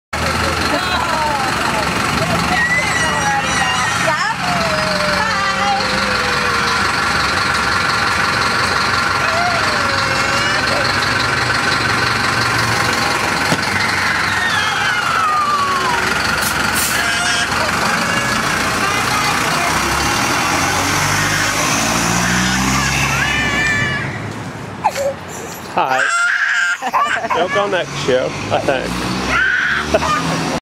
Pt 3 Clark's first day at school
Clark"s first day at preschool.

Clark has gotten so big since he was born (see earlier videos)! He is now off to preschool!